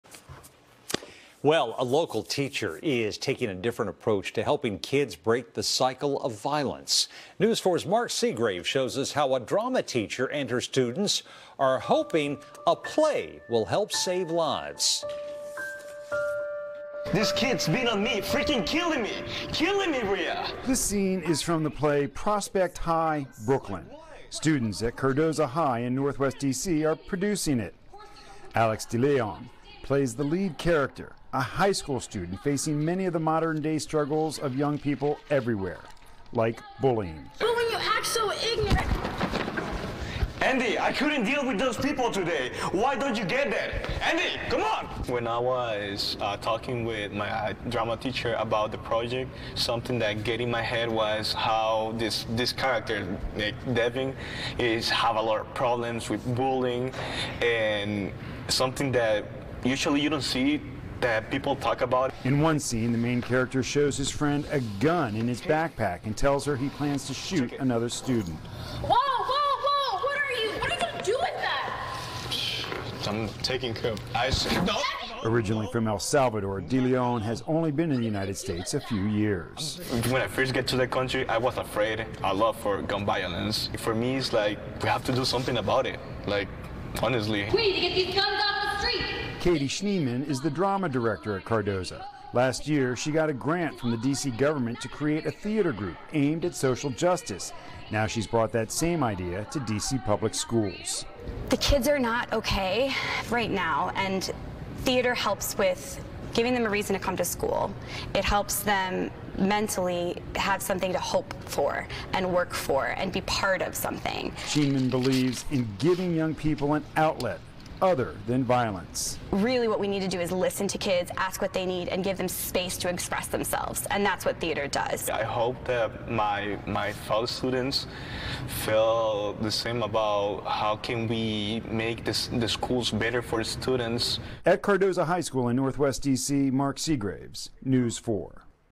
Cardozo High School Students Produce Play About Daily Struggles | NBC4 Washington
A local teacher is taking a different approach to helping kids break the cycle of violence. News4’s Mark Segraves reports.